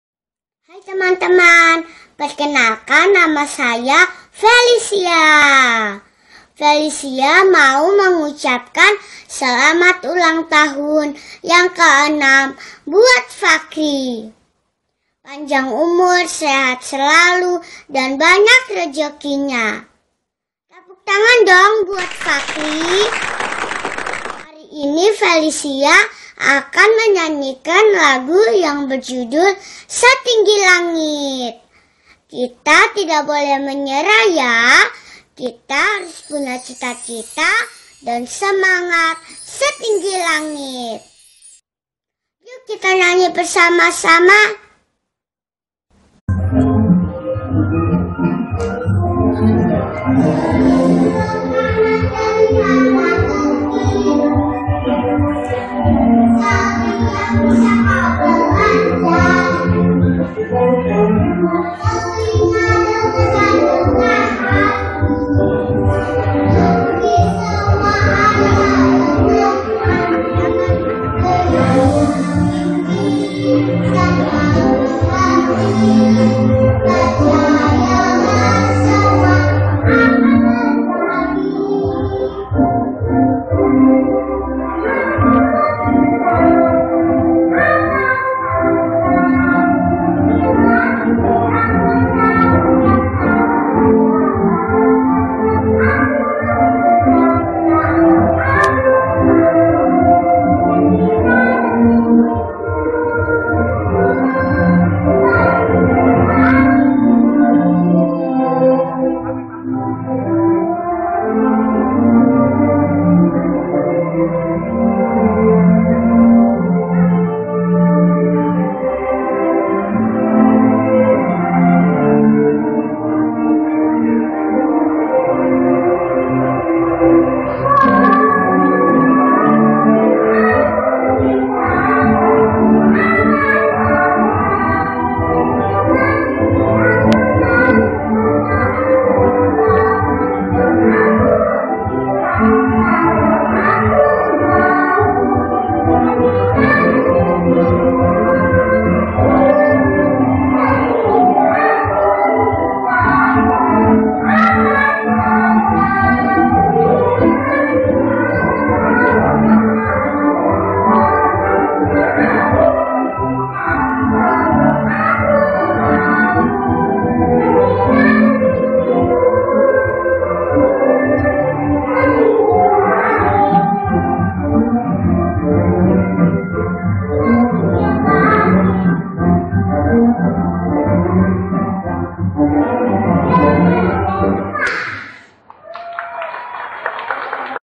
Ultah Fachrie - Performance Felicia Christine - Lagu Anak
Maaf bagian depannya ternyata agak eror. Karena mama sambil shooting, sambil diajak ngobrol teman waktu itu. Jd ga sadar kalau bagian depannya ga ke rekam. Hehehe...
Felicia bersyukur bisa ikut memeriahkan acara di HUT Fachrie yg ke 6th. Fachrie adalah littlewarrior pejuangcilik yang hebat, yg sedang melawan penyakit kanker non-hodgkins lymphoma, stadium 4. Namun Fachrie tetap ceria dan semangat. Bahkan Fachrie jago membantu ibunya memasak.
Semangat terus ya Fachrie. Kami semua senantiasa mendoakan untuk kesembuhanmu sang juniormasterchef ❤